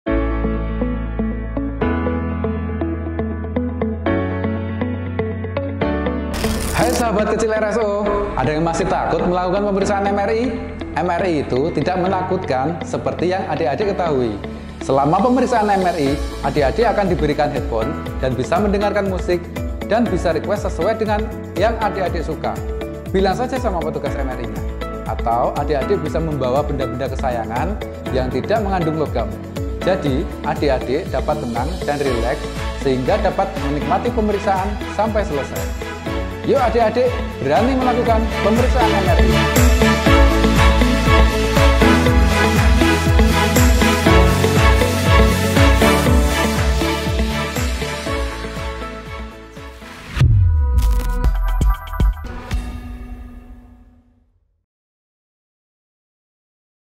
Adek-adek masih ada yang TAKUT MRI ??? | GAK USAH TAKU YA
Hallo sahabat kecil RSO
disini masih ada yang takut dengan MRI ?? gak usah takut ya adek-adek, karna MRI itu gak menakutkan lohh, bisa dengerin musik, bisa bawa barang-barang kesayangan adek-adek, misal bantal boneka dll, yang penting tidak mengandung logam.

jadi, yukk adek-adek jangan takut lagi untuk melakukan pemeriksaan MRI